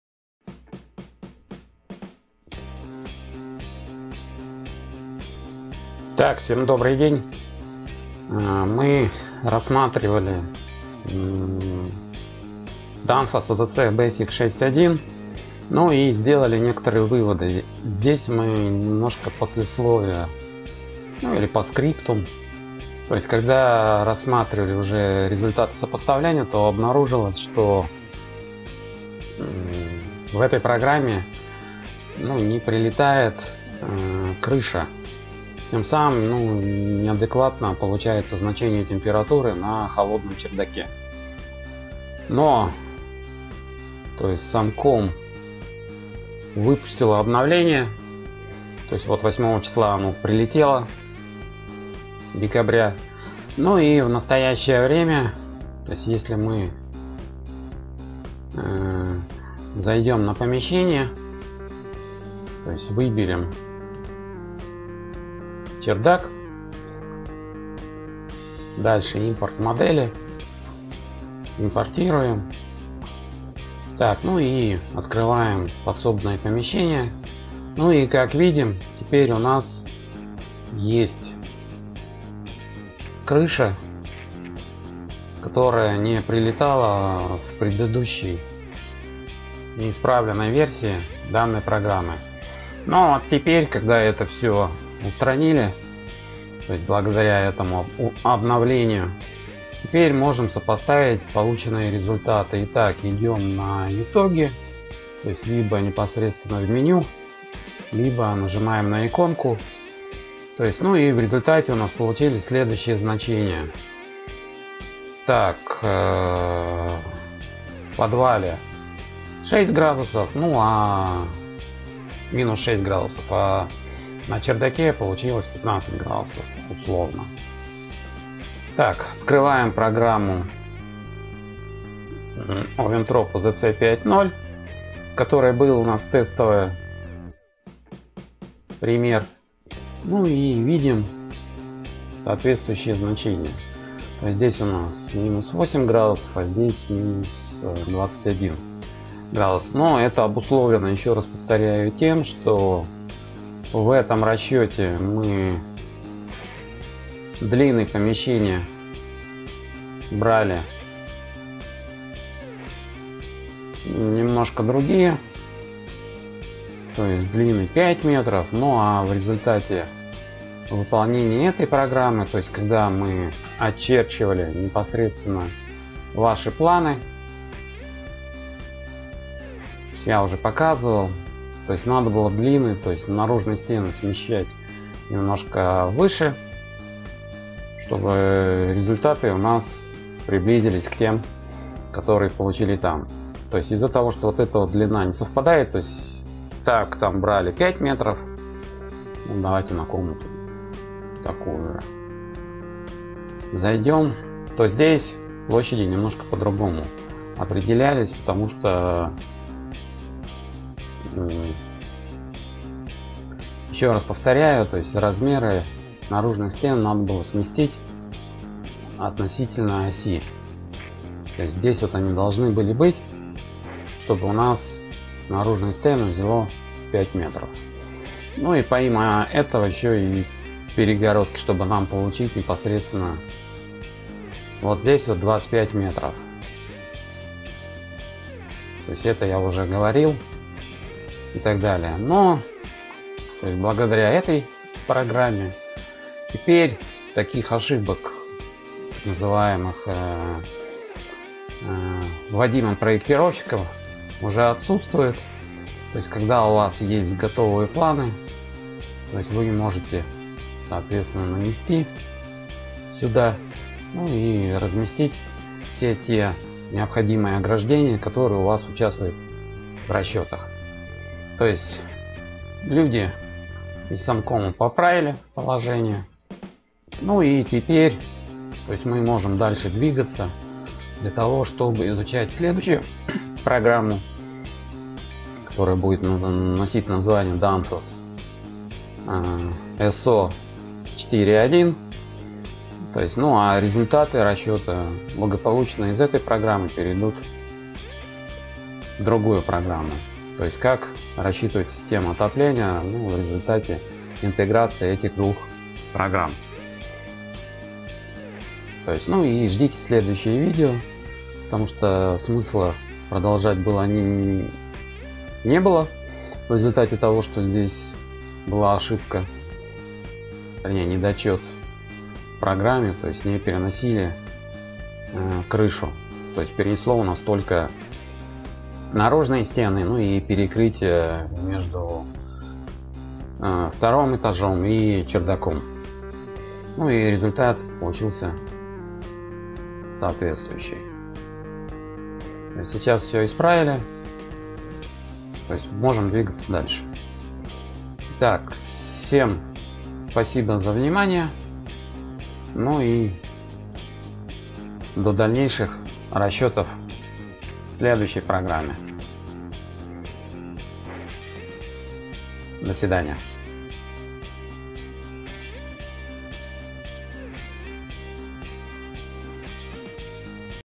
Программа Danfoss OZC 6.1. S01.E10. Итоги 2. Выводы 2.
Программа Danfoss OZC 6.1 итоги и выводы (дополнение) для лабораторной работы. Лабораторная работа по дисциплине "КТ в СТГВ" для студентов разных форм обучения. Будет полезна студентам, преподавателям и специалистам в сфере строительства.